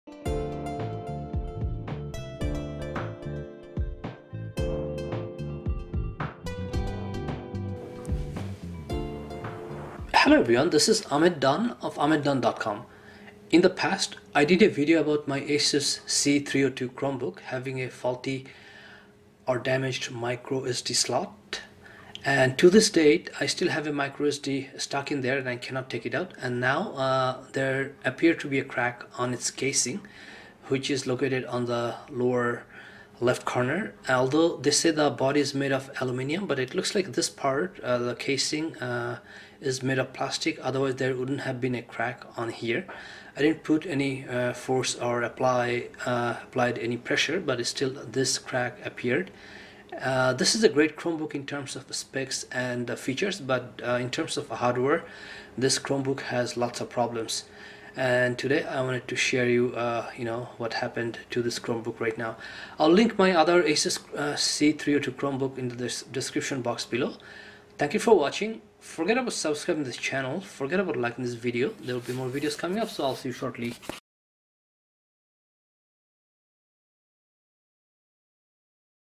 Crack on My Asus Flip C302 Chromebook | Chromebooks Tips & Tricks
Asus Chromebook Flip C302 Has A Crack

ASUS Chromebook Flip C302CA is my first Chromebook. In terms of functionality and features, Asus Flip C302 is an excellent Chromebook.

However, when it comes to hardware, Asus Flip C302 is not so good.

There appeared to be a crack on its lower end casing, next to the trackpad. Although Asus says it's made of aluminum, the casing seems to be made of plastic. Otherwise, there would not have been a crack.

In this video, I will show exactly what happened. Also, the same  Asus Flip C302 Chromebook ate my Micro SD card and has a damaged SD slot.